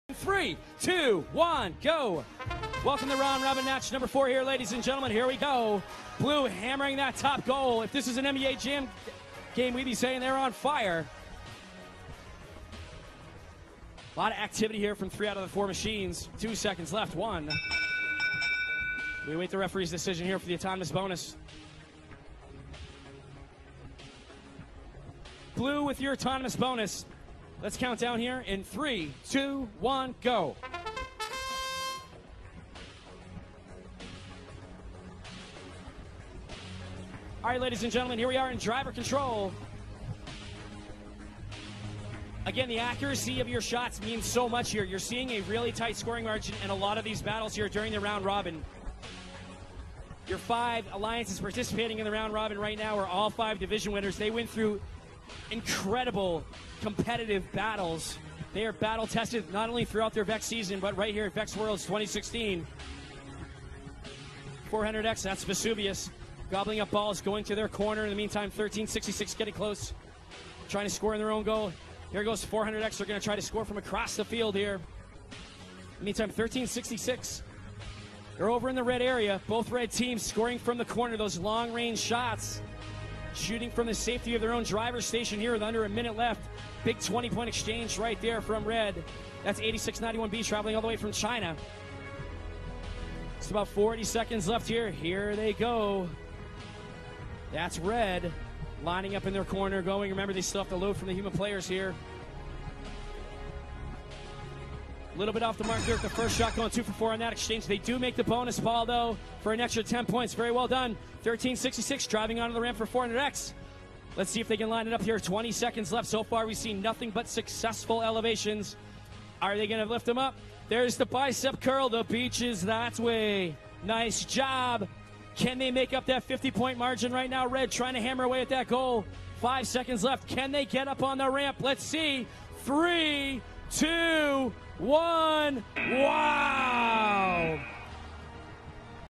VEX Worlds 2016 - VRC HS Finals - Round Robin 4 (8691E 1104M 400S) 233 vs 287 (1366 400X 2105C)
VEX Worlds 2016 - VRC HS Finals - Round Robin 4

Red - Score 233:
8691E - hefei NO.8 SMSch E (Hefei No.8 Senior Middle School, hefei, China)
||  Technology Rank:1 . Wins:10 . Loses:0  . WP:20 . SP:1465

1104M - Discobots ? (Brampton Robotics Education, Brampton, Ontario, Canada)
||  Technology Rank:30 . Wins:7 . Loses:3  . WP:14 . SP:1045

400S - Galveston I.S.D. Robotics (Galveston I.S.D. Robotics, Galveston, Texas, United States)
||  Technology Rank:22 . Wins:7 . Loses:3  . WP:14 . SP:1474

Blue - Score 287:
1366 - Berkner Fearless Robotics (Berkner STEM Academy, Richardson, Texas, United States)

400X - Floyd IV - Vesuvius (Galveston I.S.D. Robotics, Galveston, Texas, United States)

2105C - Swampbotics (Ware County High School, Waycross, Georgia, United States)